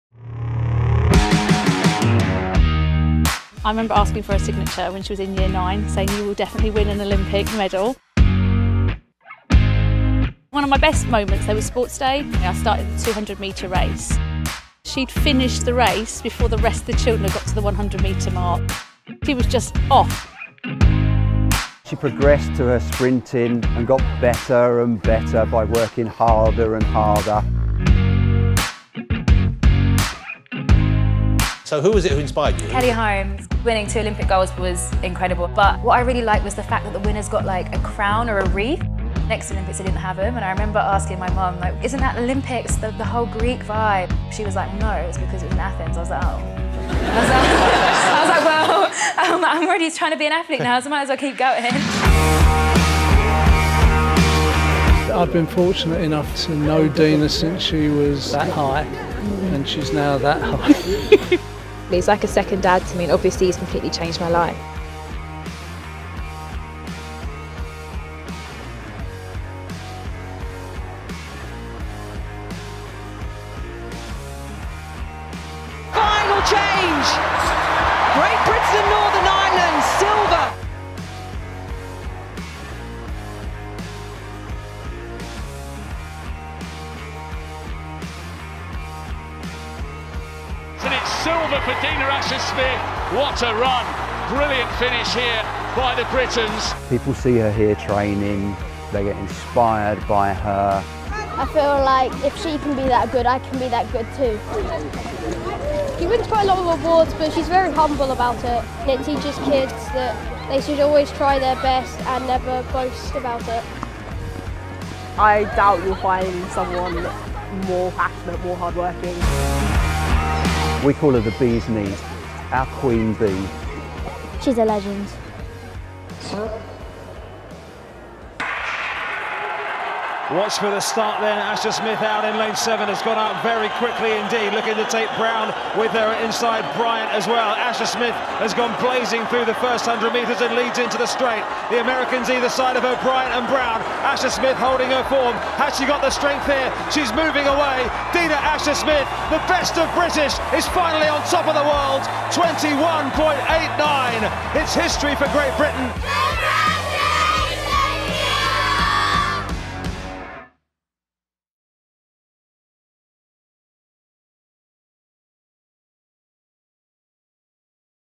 You'll never guess who was inspired by Dame Kelly Holmes at the age of 15 to start running ...
15 years ago, a little girl was so inspired by watching Kelly Holmes win double Olympic gold in Athens in 2004 that she asked her mum if she could have a go at athletics. Her mum took her to the Bromley Bees athletics club in south east London – and the rest as they say is history!

Our 2019 Sporting Hero Award winner is the new 200m world champion, Dina Asher-Smith